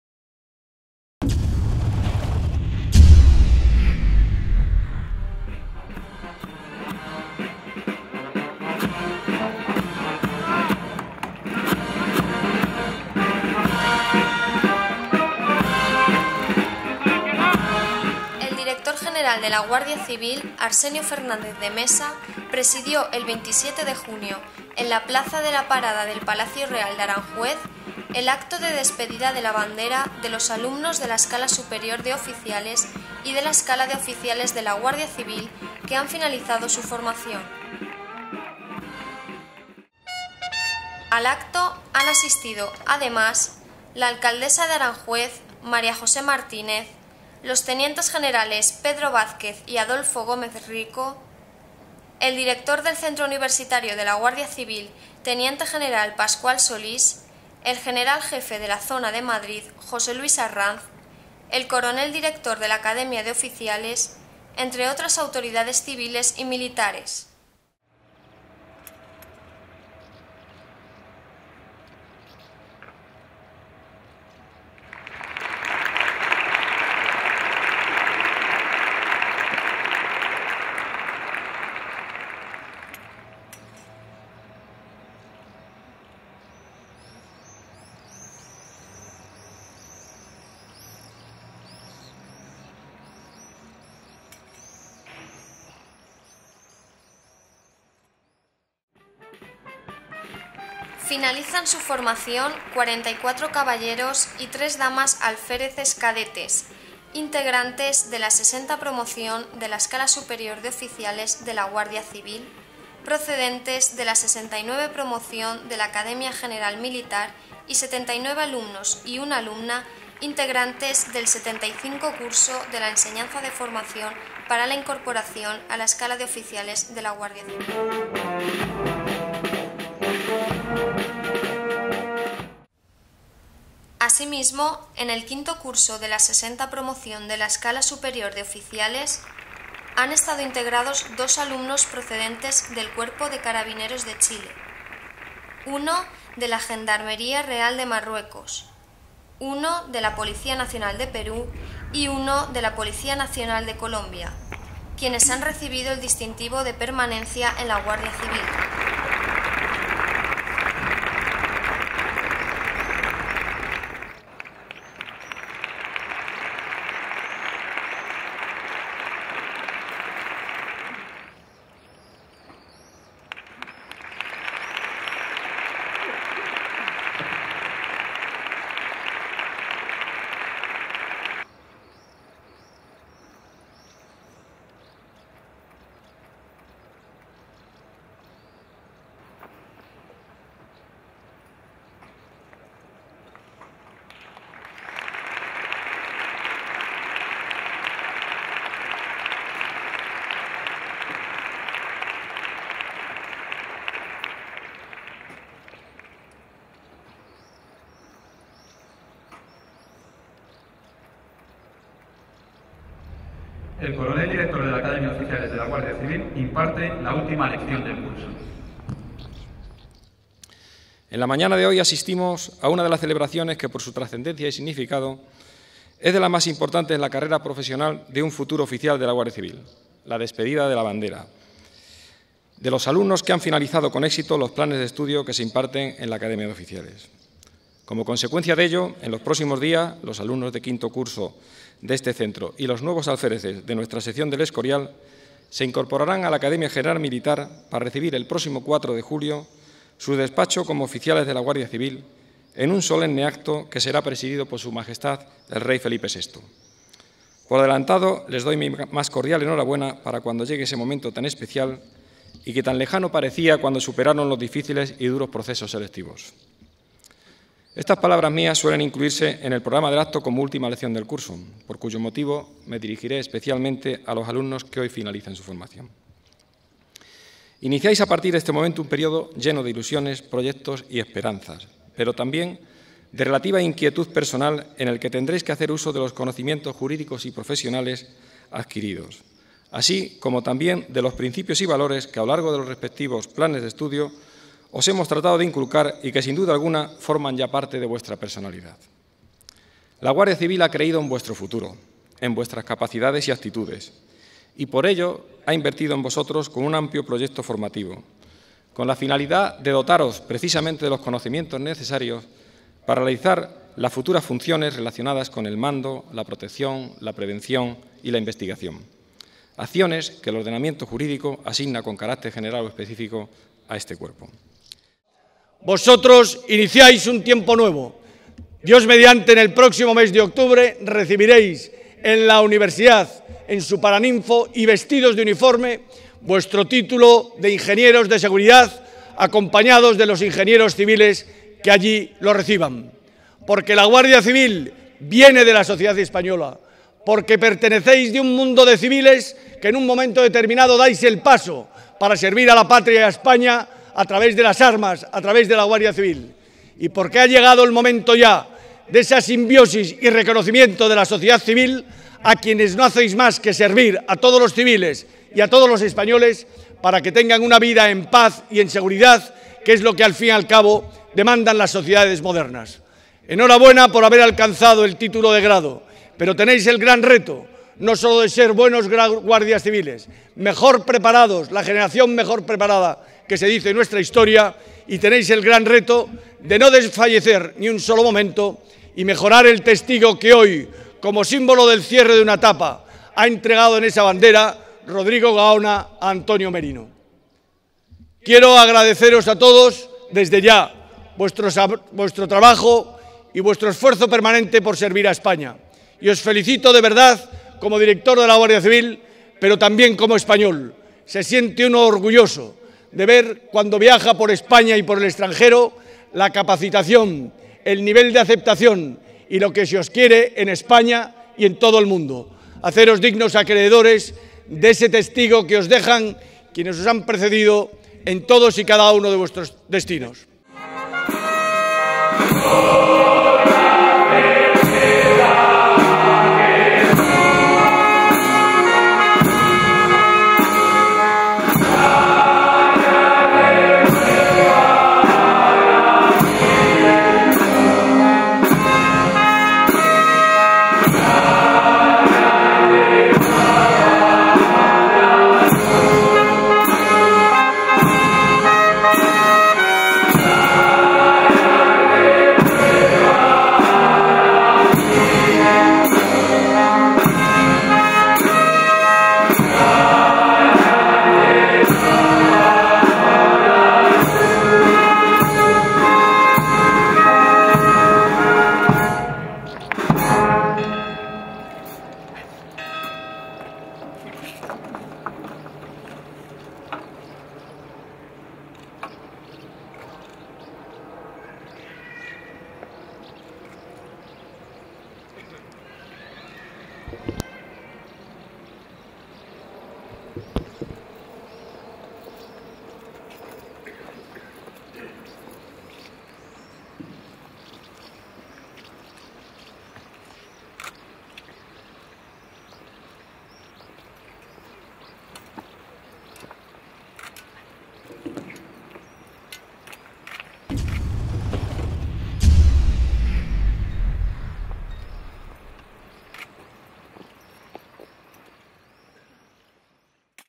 El Director General preside el acto de despedida de la bandera de los Oficiales en Aranjuez
El Director General de la Guardia Civil, Arsenio Fernández de Mesa, presidió el pasado 27 de junio, en La Plaza de la Parada del Palacio Real de Aranjuez (Madrid), el acto de despedida de la bandera de los alumnos de la Escala Superior de Oficiales y de la Escala de Oficiales de la Guardia Civil que han finalizado su formación.

Al acto han asistido además la Alcaldesa de Aranjuez, María José Martínez;  los tenientes generales Pedro Vázquez y Adolfo Gómez-Rico, el Director del Centro Universitario de la Guardia Civil, Teniente General Pascual Solís; el General Jefe de la Zona de Madrid, José Luis Arranz; el Coronel Director de la Academia de Oficiales, entre otras autoridades civiles y militares.

En el acto de despedida de la bandera, el Director General ha hecho entrega de sendas distinciones a los números uno de cada promoción de los oficiales que acceden a la Escala Superior de Oficiales, el Alférez Cadete Rodrigo Gaona y de la Escala de Oficiales de la Guardia Civil,  Rosa María Touris,  una vez concluido su periodo de formación.

Finalizan su formación 44 Caballeros y 3 Damas Alféreces Cadetes, integrantes de la 60 Promoción de la Escala Superior de Oficiales de la Guardia Civil, procedentes de la 69 Promoción de la Academia General Militar y 79 alumnos y 1 alumna, integrantes del LXXV Curso de la Enseñanza de Formación para la incorporación a la Escala de Oficiales de la Guardia Civil.

Asimismo, en el 5º Curso de la 60 Promoción de la Escala Superior de Oficiales, han estado integrados dos alumnos procedentes del Cuerpo de Carabineros de Chile, uno de la Gendarmería Real de Marruecos, uno de la Policía Nacional de Perú y uno de la Policía Nacional de Colombia quienes han recibido el distintivo de permanencia en la Guardia Civil.

Arsenio Fernández de Mesa ha comenzado sus palabras destacando que este acto es de una enorme importancia para la Academia de Oficiales, para la Guardia Civil y para España. Ha continuado felicitando a todos los alumnos de 5 curso de la Escala Superior de Oficiales, a los que le ha dicho que cierran un ciclo de formación en la Guardia Civil, ya que son los últimos del antiguo plan de estudios, puesto que los que salgan con el Empleo de Teniente el próximo año, lo harán también por primera vez con el Grado de Ingenieros en Seguridad.

El Director General ha destacado que sobre ellos recae la responsabilidad y el orgullo de dirigir el mejor activo que tiene este Instituto: sus guardias civiles. Sed conscientes de que ellos, junto con el resto de la sociedad, esperan mucho de vosotros y debéis corresponderles colmando plenamente sus expectativas.

Asimismo el Director General les ha transmitido a los alumnos que hoy terminan su formación como Oficiales que tienen la responsabilidad de mejorar lo que han recibido de sus predecesores y de agrandarlo para los que vendrán detrás de ellos. Valores como el compañerismo, el afecto y el respeto tanto a vuestros jefes como a vuestros subordinados son valores que Arsenio Fernández de Mesa ha destacado como esenciales, que unidos a una excelente formación, han de ser los pilares sobre los que se asiente su trabajo diario que van a comenzar en dentro de la Guardia Civil.

A modo de conclusión el Director General les ha felicitado a todos por el esfuerzo y el trabajo que han hecho durante su periodo formativo y les ha dicho que tienen ante ellos el reto de mejorar la Guardia Civil, ya que son los mejores preparados de la historia. Os felicito como Director General y como español que se siente orgulloso de un Cuerpo que es el más valorado por los españoles y al que se le reconoce cada vez más en el ámbito internacional.

También ha tenido unas palabras de agradecimiento a los alumnos de otros cuerpos policiales que han cursado sus estudios en la Academia y al Comandante de la Gendarmería Nacional Francesa, Stephane Jean Caille, que con motivo de su ascenso deja la Academia donde ha sido profesor durante 4 años.

Para finalizar, Arsenio Fernández de Mesa ha tenido unas palabras de recuerdo emocionado para todos los guardias civiles que han fallecido en acto de servicio, especialmente las víctimas de la sin razón de la barbarie terrorista.

Sus últimas palabras han sido de reconocimiento a SM El Rey Don Juan Carlos, de quien ha destacado su impagable trabajo al servicio de España y su enorme generosidad al haber abdicado a favor de su hijo SM El Rey Felipe VI, quien dará continuidad a la Monarquía.